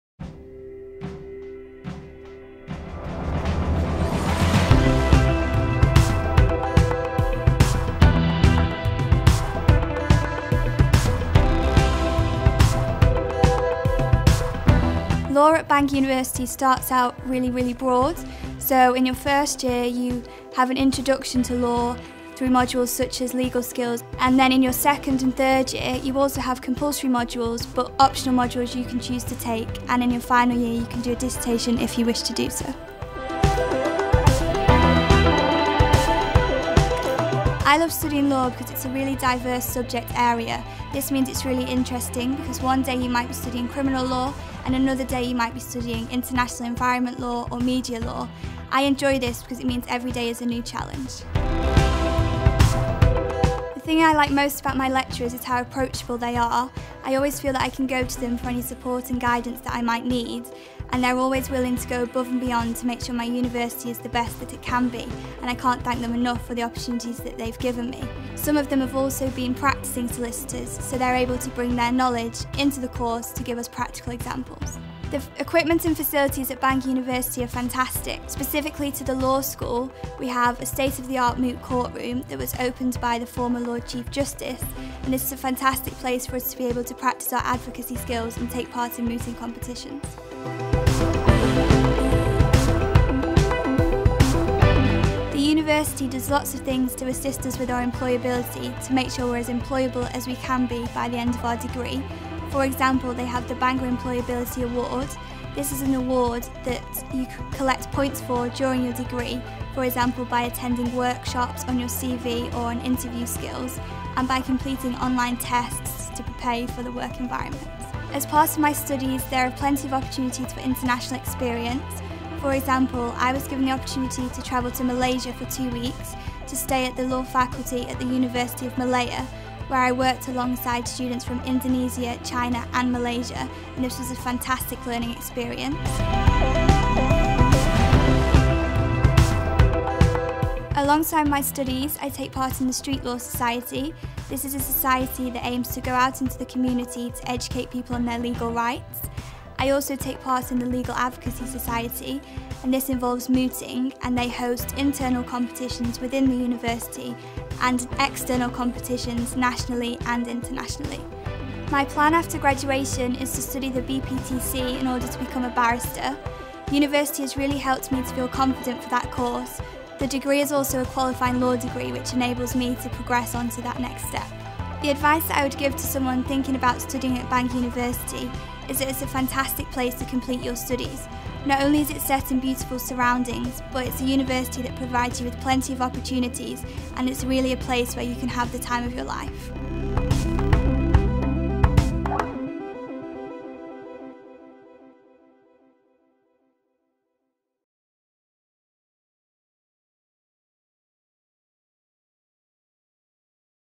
Studying Law
The Law degrees offered by Bangor Law School have been carefully designed to evolve with the ever-changing legal landscape, and to equip students with the legal knowledge and skills demanded by an increasingly globalised industry.

Our small class sizes foster a lively learning environment and encourage students to engage fully with the teaching experience, with students and lecturers developing a close and supportive relationship.